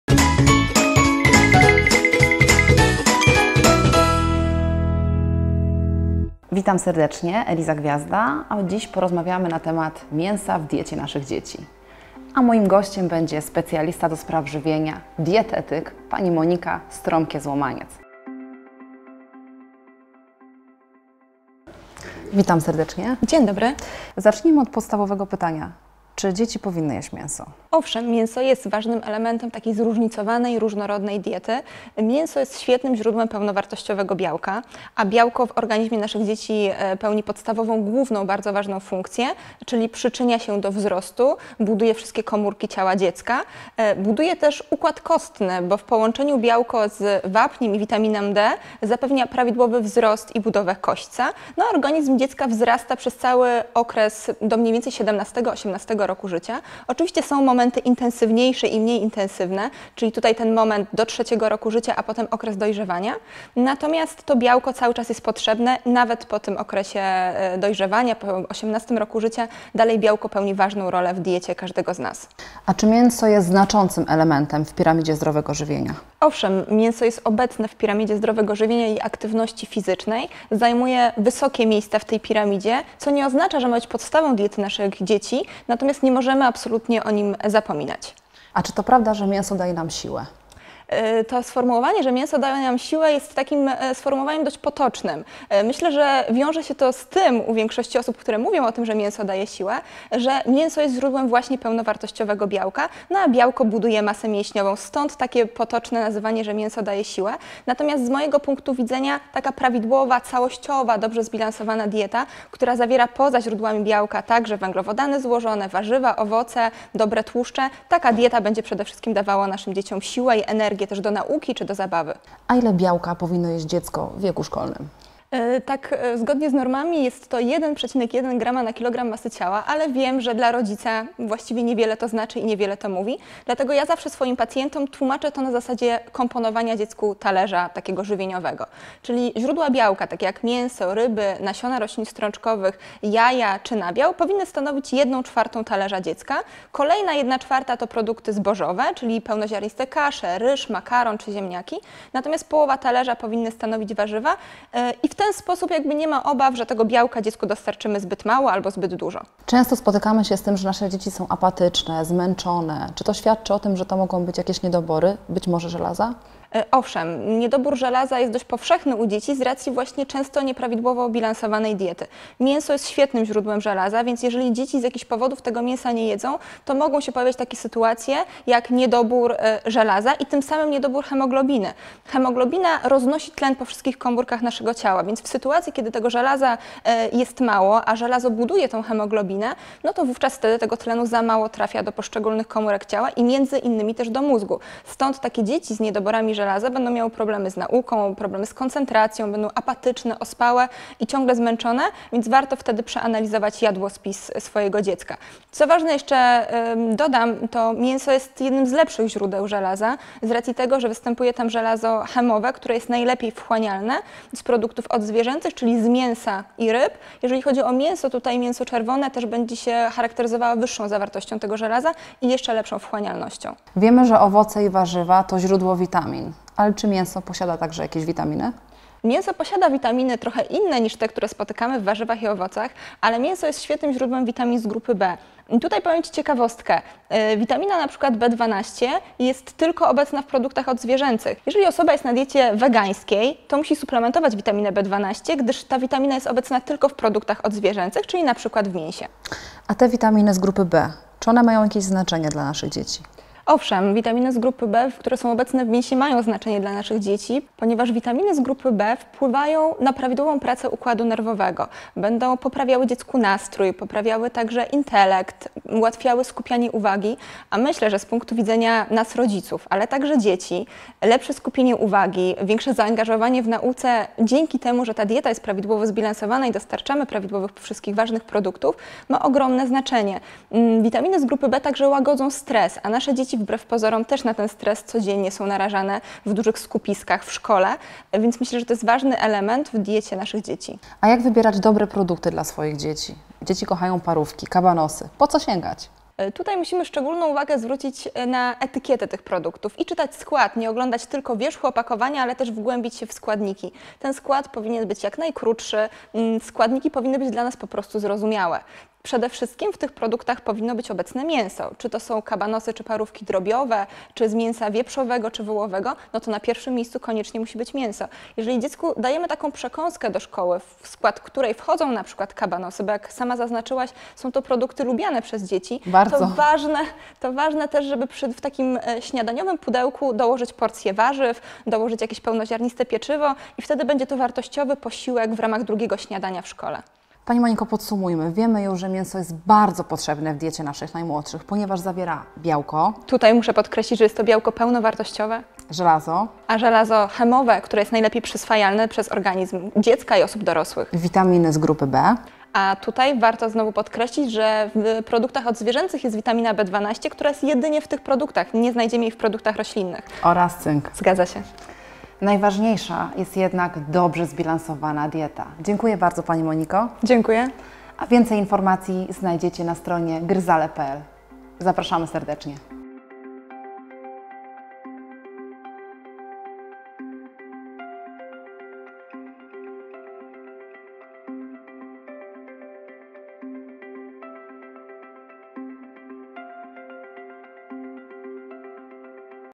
Rola mięsa w diecie dziecka
Czy mięso jest dzieciom potrzebne? Jaką odgrywa rolę? Ile mięsa powinno spożywać dziecko?
Zobacz nasz wywiad z ekspertem i dowiedz się, jak dbać o prawidłową, zbilansowaną dietę dziecka.

Wywiadu udzieliła dietetyk, mgr Monika Stromkie-Złomaniec.

Dowiedz się więcej o tym, jak wspierać prawidłowy rozwój dziecka na